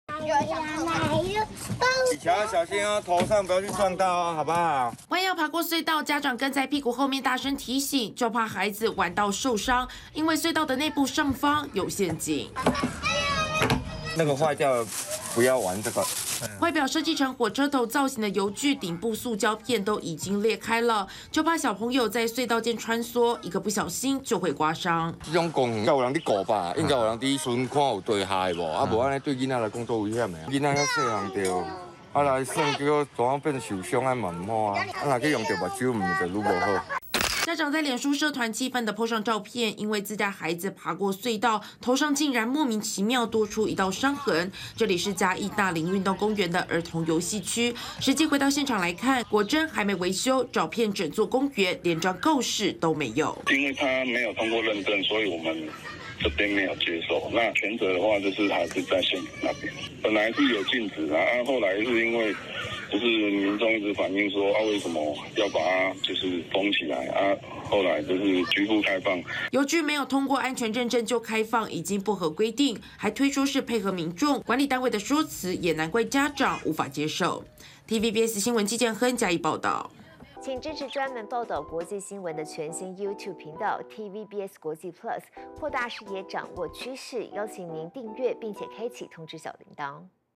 兒童設施藏陷阱！遊具塑膠片破裂 孩童頭部遭刮傷｜TVBS新聞@TVBSNEWS01
有家長反應，帶孩子到嘉義的大林-運動公園玩，兒童遊樂設施-出現損壞，小朋友爬過隧道，頭頂卻被裂開的塑膠片刮傷，根據公所的說法，這座公園還沒通過安全認證，因此照理來說不能開放，是應家長們敲碗要求，才開放局部空間，不過這一番說詞，實在讓家長們難以接受。